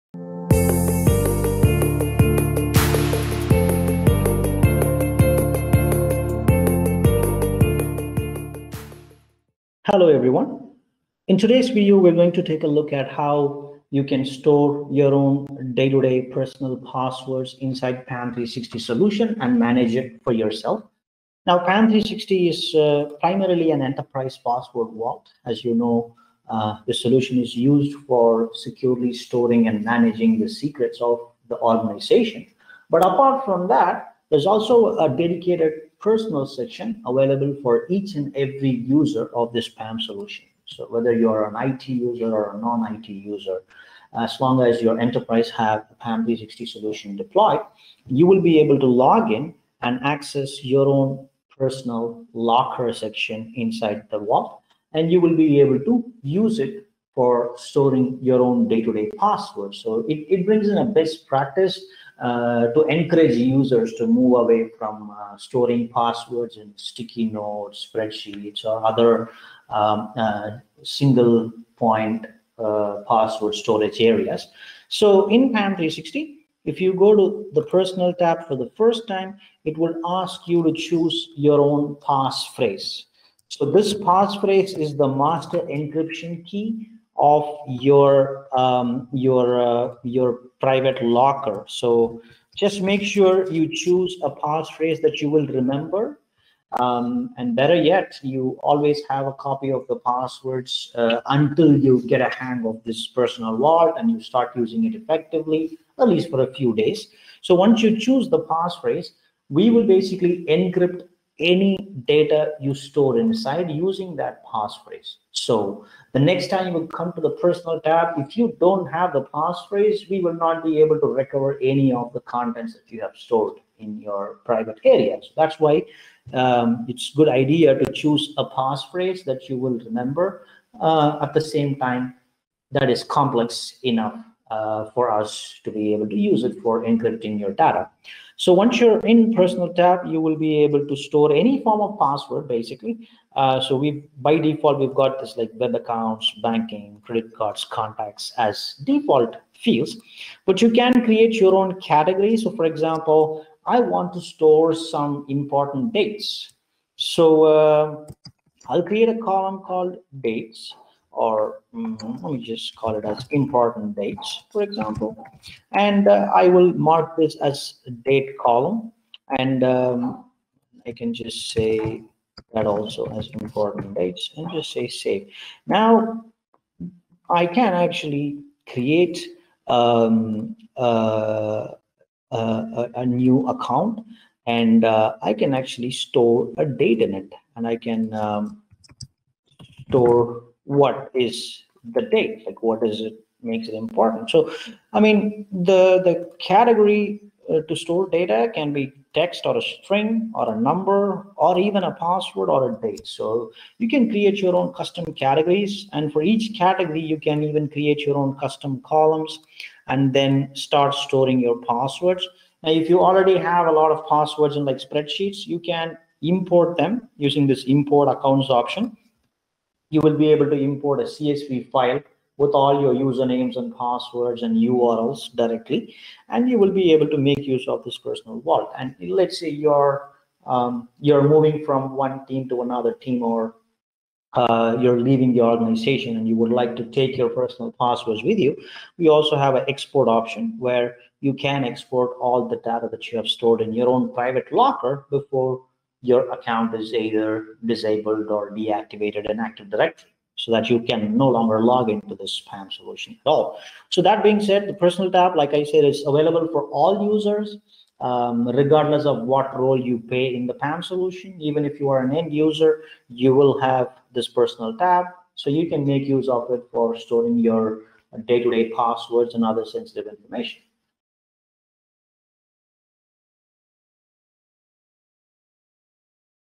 Managing personal passwords | ManageEngine PAM360
ManageEngine PAM360 is an enterprise-grade unified privileged access management solution that offers strict governance of privileged accounts, credentials, digital entities, and other shared sensitive information. Watch this tutorial on how to securely store, track and manage personal passwords in PAM360.

You can also try out the fully functional online demo to see what you get,